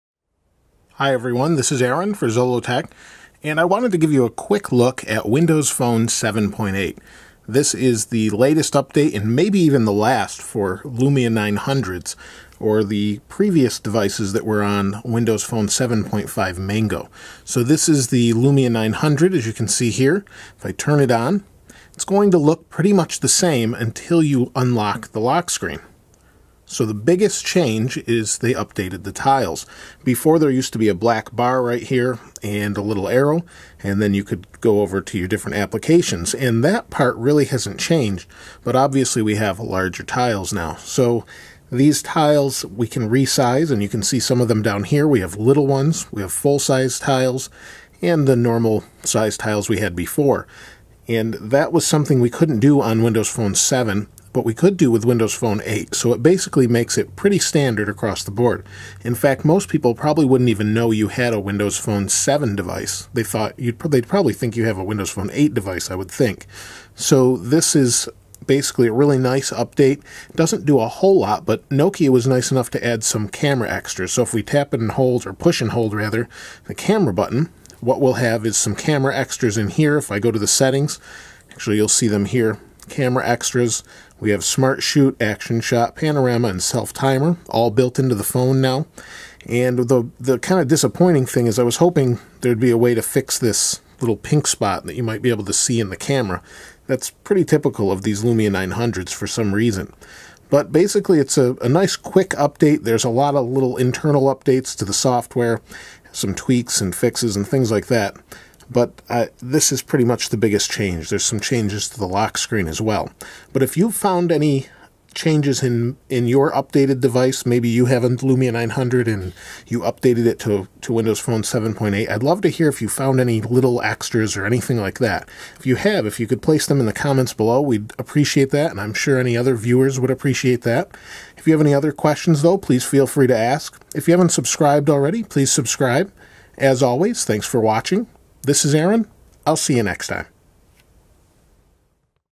Windows Phone 7.8 Update Overview
Windows Phone 8 has been out for a few months and has separated itself pretty drastically from the less than one year old Windows Phone 7.5.  In order to bring everything to the new look of WP8, Microsoft had promised an update for those left behind early devices to Windows Phone 7.8.  It's been a long time coming and today Nokia has made WP7.8 available for download on all Lumia 800, 900 and lower end Lumia devices.

We take a quick look at what's new and help you decide if you even want to update.